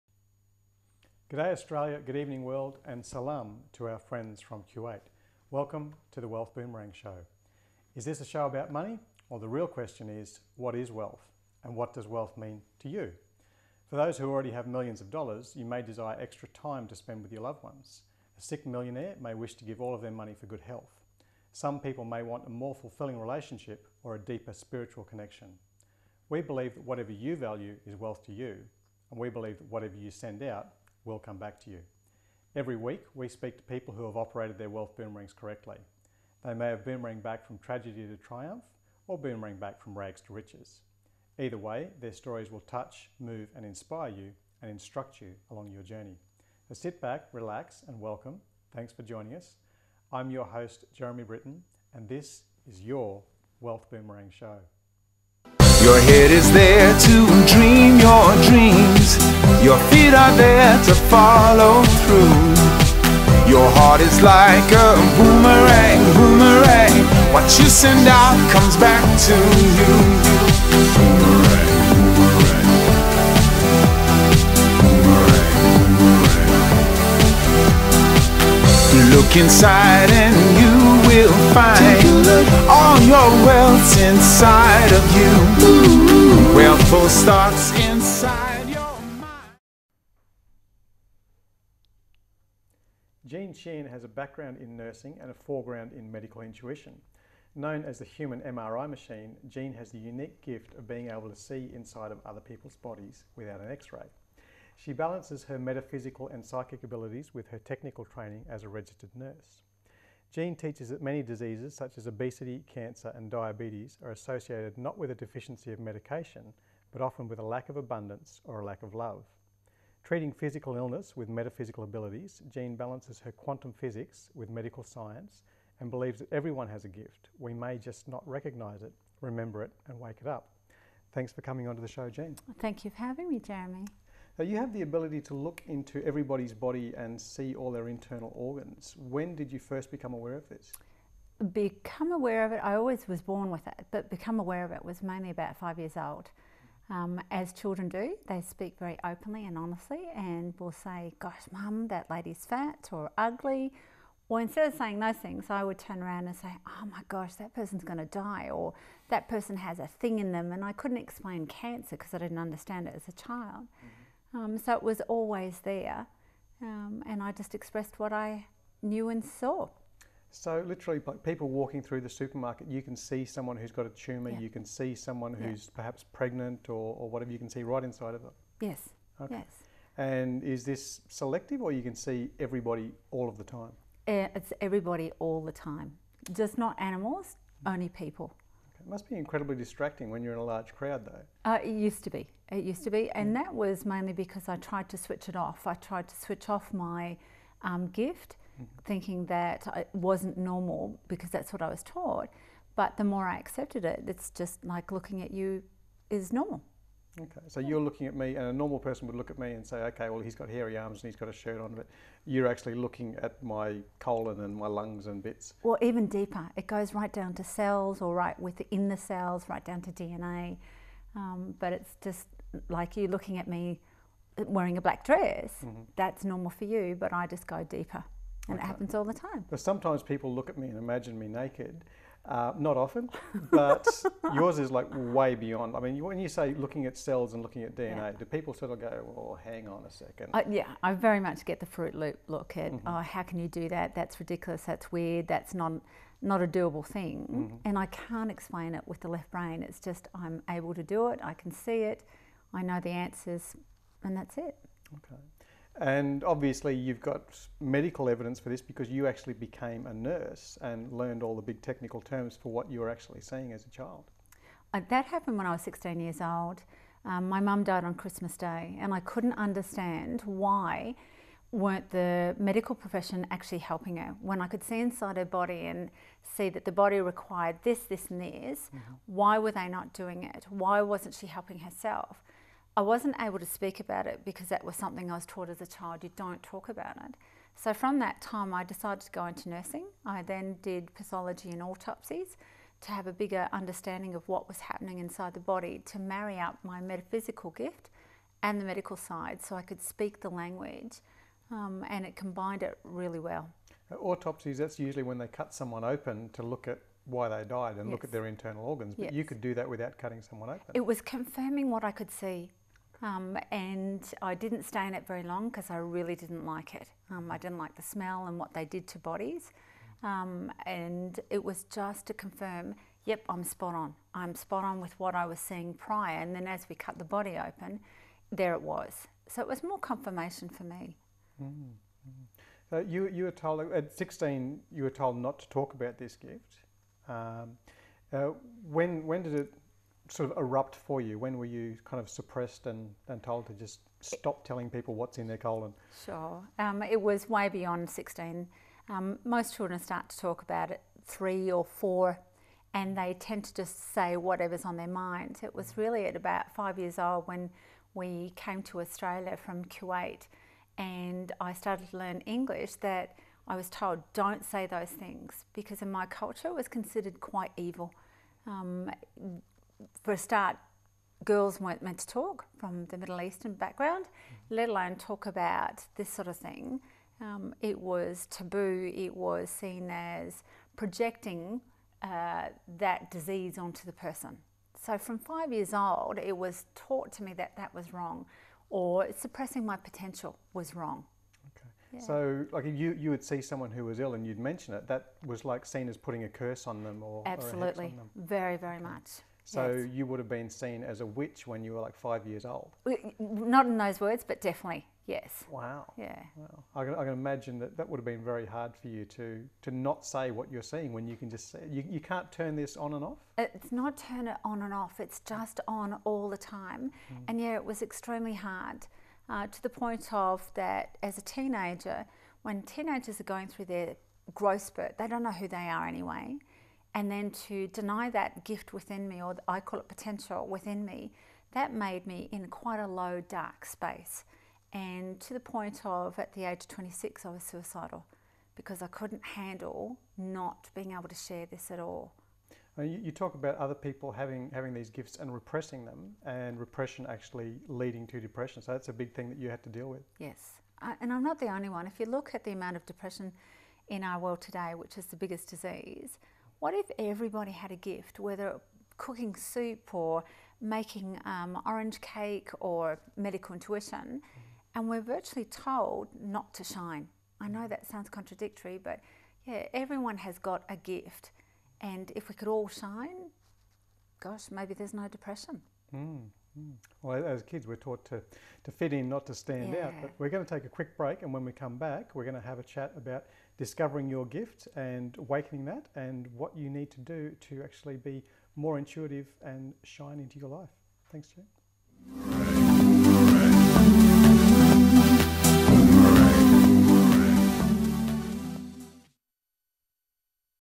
Wealth Boomerang Show #5 Part 1
Jean Sheehan, the "Human MRI" discusses being persecuted for her gifts